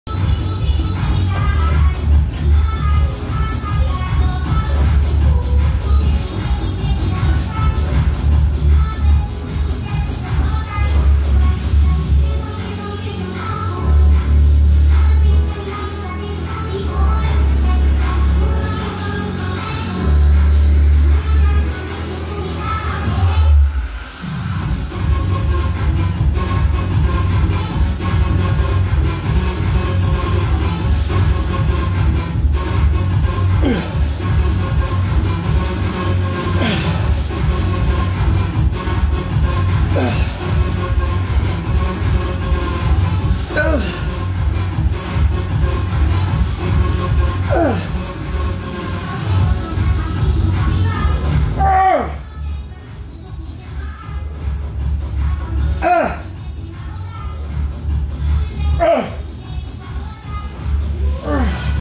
Michael Gundill fait les biceps dans une position à la mord moi le nœud
Michael Gundill fait des curls biceps d'un bras, allongé, à la poulie mi-basse